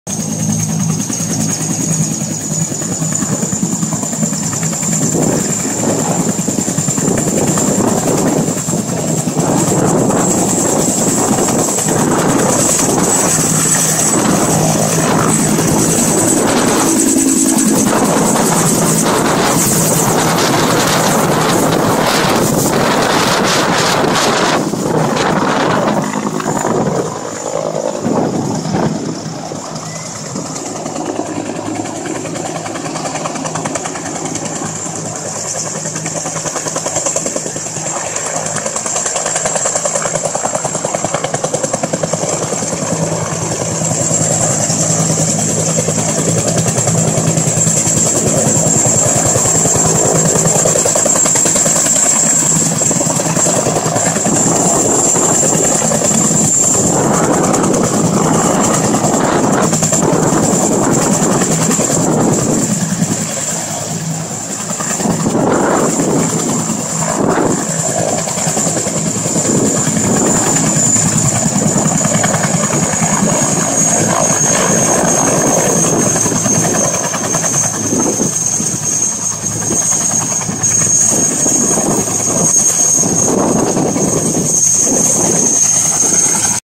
Helitac's Bell 214B "swapping" positions to refuel on Glen Forrest oval during a Perth bushfire.
Two Bell 214B helitac's "swap" positions on Glen Forrest Oval to refuel. The oval was used as a refuel point during a Perth bushfire in Mt Helena on 16th February 2014. The Bell 214B is the most powerful (2950 SHP) single engine helicopter in the world, fitted with the Isolair 'Tsunami" Belly Tank, with 2,700 Litres capacity.